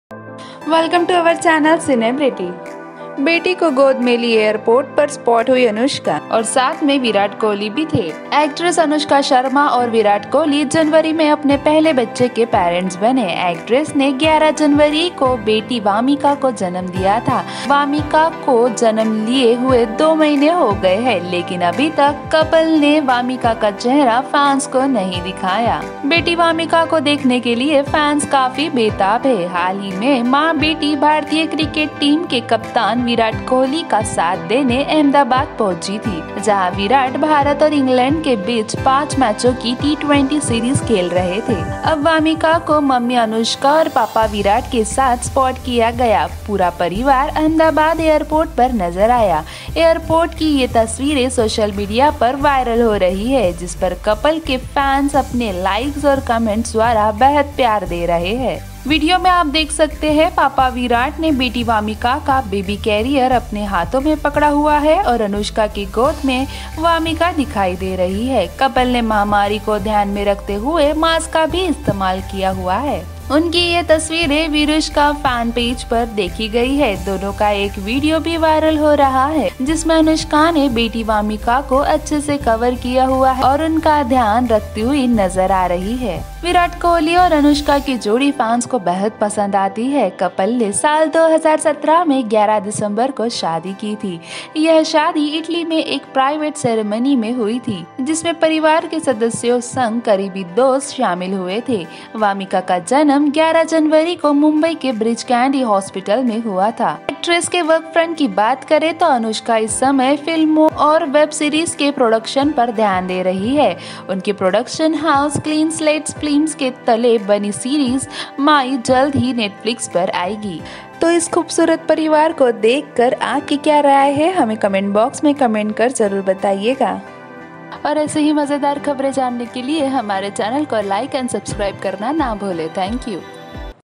Virat Kohli & Anushka Sharma Spotted With Daughter Vamika At Airport | Cinebrity
Watch Virat Kohli & Anushka Sharma Spotted With Daughter Vamika At Airport | Cinebrity